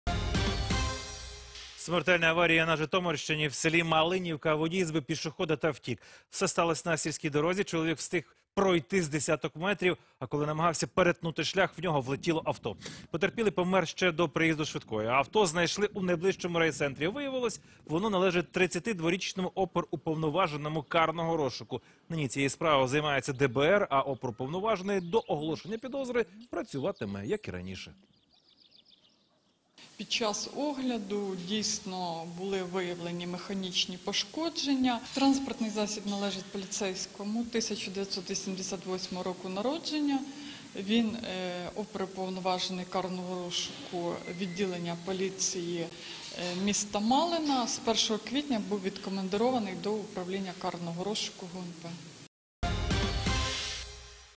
Смертельна аварія на Житомирщині: у селі Малинівка водій збив пішохода та втік
Усе сталось на сільській дорозі: чоловік встиг пройти десять метрів, а коли намагався перетнути шлях, то у нього влетіло авто. Потерпілий помер ще до приїзду швидкої. Авто знайшли у найближчому райцентрі. Воно належить 32-річному оперуповноваженому карного розшуку.